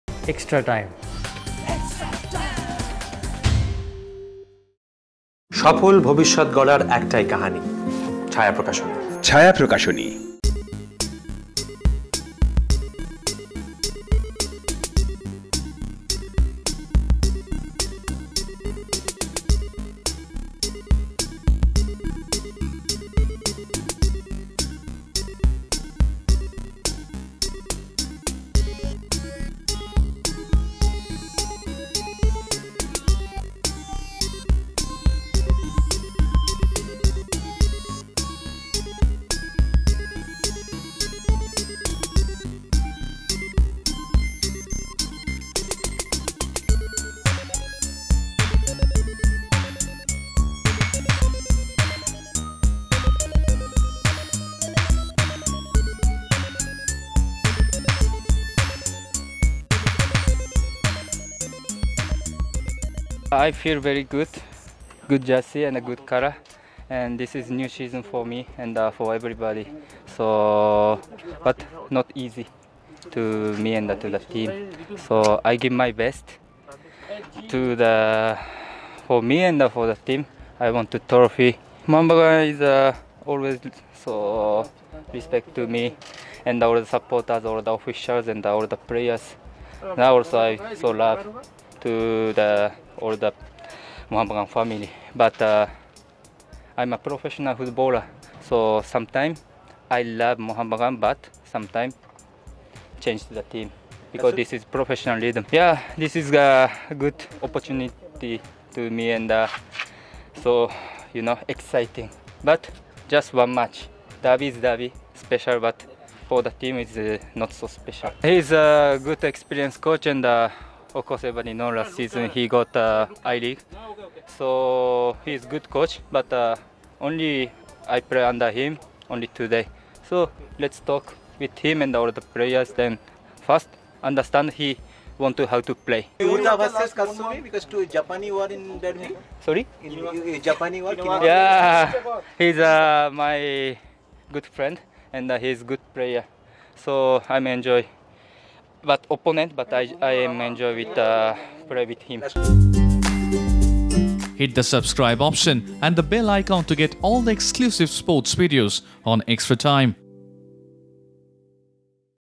Watch: Katsumi Yusa's first day at East Bengal club
Japanese Katsumi Yusa hogged the limelight during East Bengal training session today at the club ground. Katsumi, who donned Mohun Bagan jersey last season, has decided to switch over to East Bengal.  The attacking midfielder was the star attraction today at East Bengal club as journalists and photographers from his native Japan was also seen covering his practice. The 29 year old was in a jovial mood during his first training session under Coach Khalid Jamil.